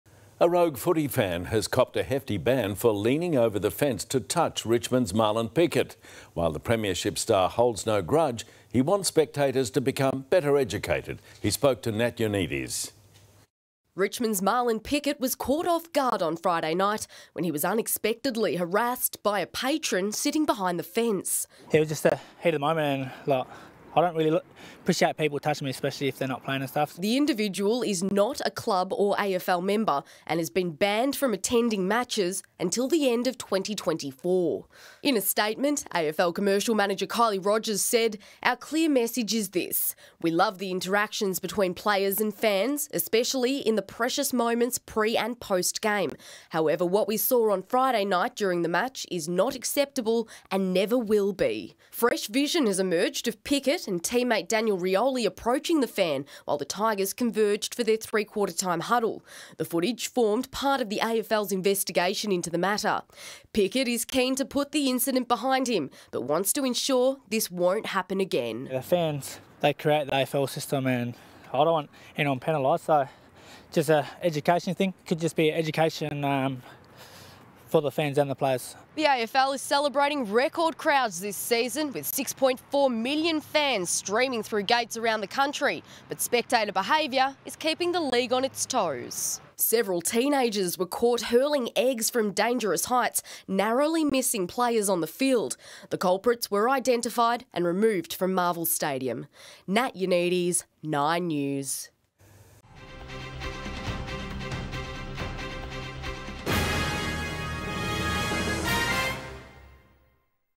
Footy fan cops hefty ban for touching AFL player during game | 9 News Australia
While the Premiership star holds no grudge, he wants spectators to become better educated.